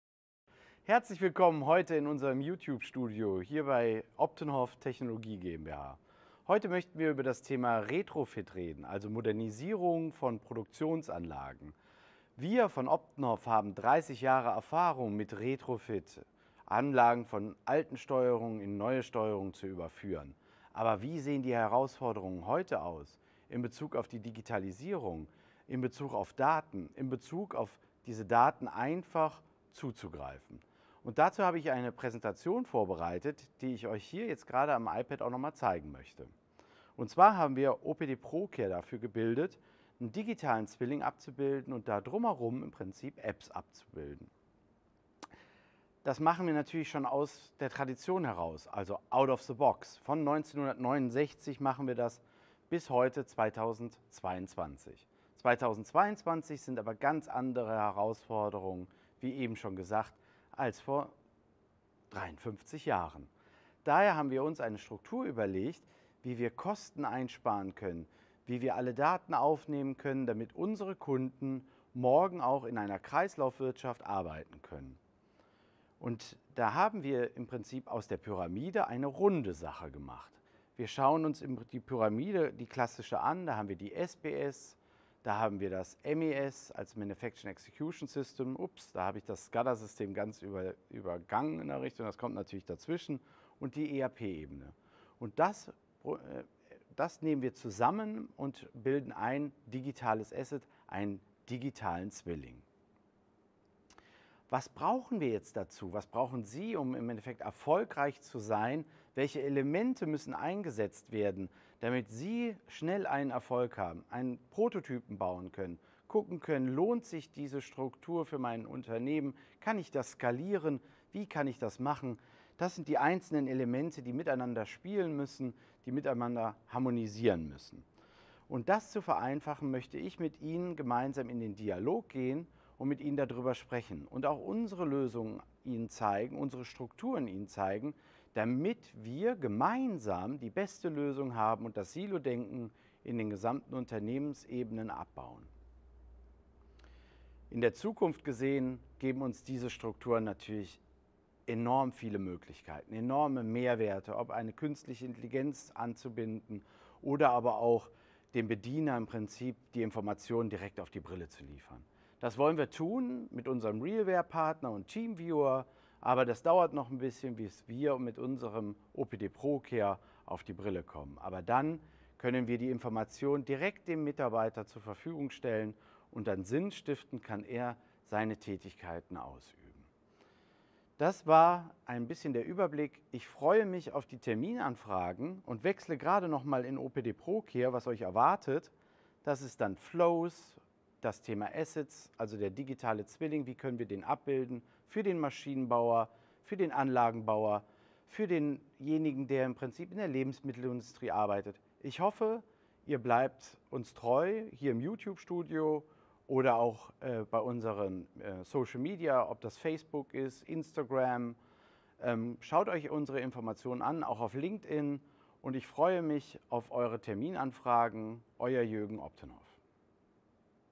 OPDENHOFF - Retrofit, Digitalisierung & OPDPro.Care
Mit über fünf Jahrzehnten Erfahrung, zeigen wir heute die Möglichkeiten im Retrofit und das Überführen von alten Steuerungen in neue Steuerungen für Bestandsanlagen. Digitalisierung bietet viele neue effiziente Lösungen, zusammen mit OPDPro.Care schaffen wir dafür die Grundlage in Ihrer Produktion. OPDPro.Care schafft eine Struktur für Kostenersparnis, Aufnehmen von Daten aus Produktion und vielen weiteren Mehrwerten damit unsere Kunden auch Morgen in einer Kreislaufwirtschaft Arbeiten können.